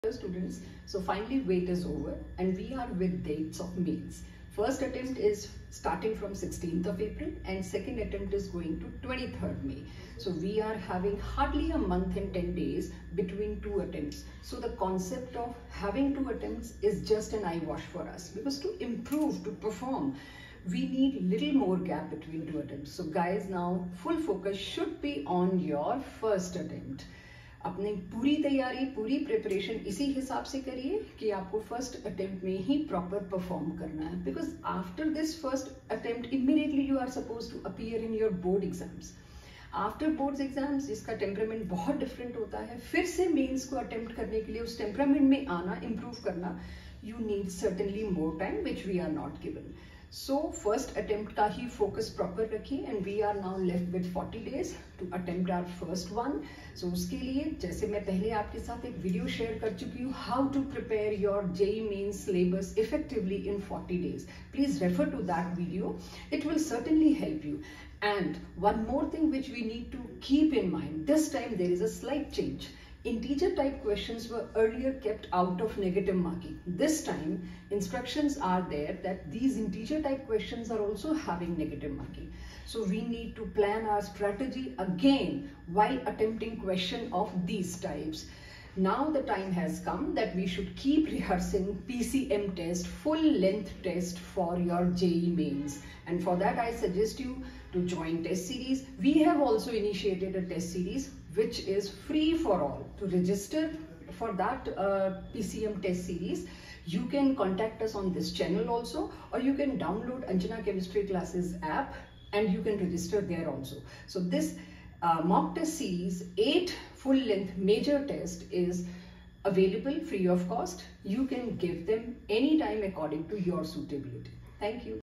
JEE Mains dates 2022 announced . Learn important ways to get good rank and how to prepare now .
JEE Mains 2022 dates announced today . This in continuation to earlier videos about how to prepare and what to focus .